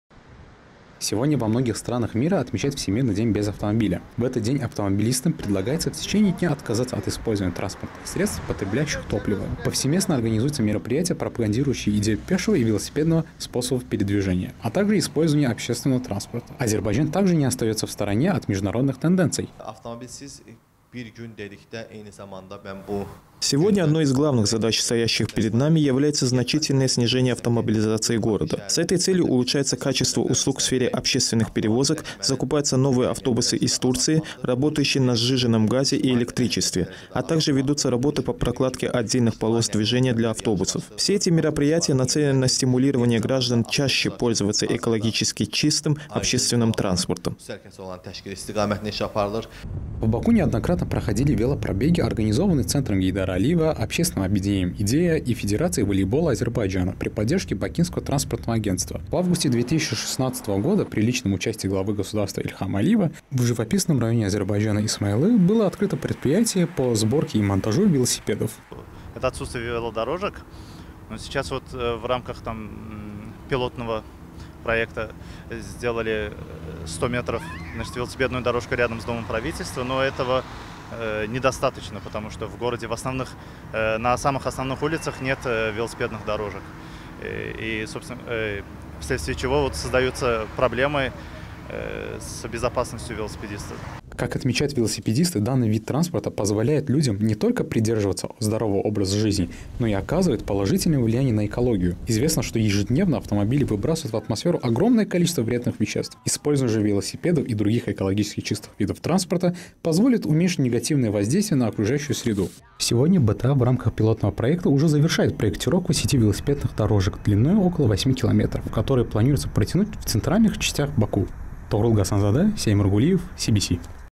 Всемирный день без автомобиля
Сегодня во многих странах мира отмечают Всемирный день без автомобиля. В этот день автомобилистам предлагается отказаться от использования транспортных средств, потребляющих топливо. Повсеместно организуются мероприятия, пропагандирующие идею пешего и велосипедного способов передвижения, а также использование общественного транспорта. Азербайджан также не остается в стороне от международных тенденций.